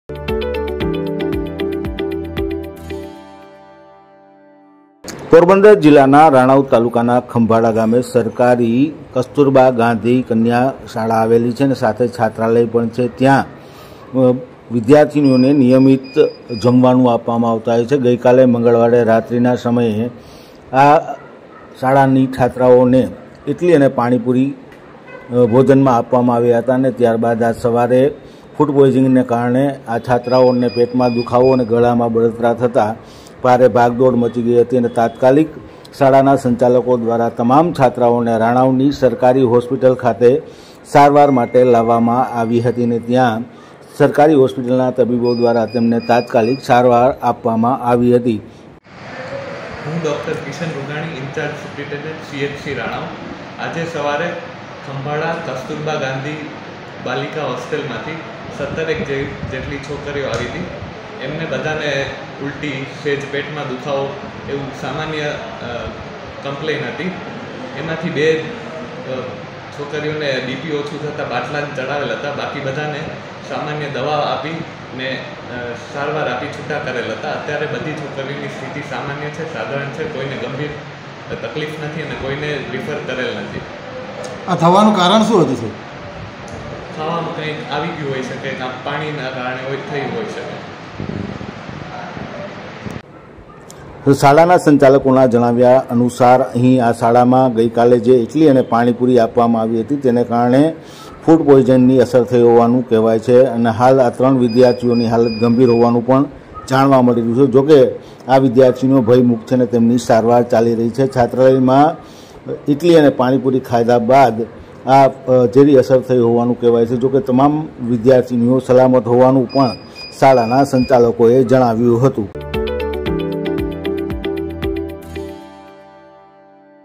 પોરબંદરના ખંભાળા ગામે વિદ્યાર્થીનીઓને ઝેરી અસર
શાળામાં ફુડ પોઇઝનીગની ઘટના
ખંભાળાની કસ્તુરબા ગાંધી કન્યા છાત્રાલયમાં બનાવ
21 છાત્રાઓને ફુડ પોઇઝનીંગની અસર થતાં સારવારમાં
ગત રાત્રીના પાણીપુરી અને ઇટલી ખાધા બાદ ઝેરી અસર
સારવાર માટે રાણવાવની સરકારી હોસ્પિટલ ખાતે લાવામાં આવી
તમાસ છાત્રાની તબિયત સુધારા પર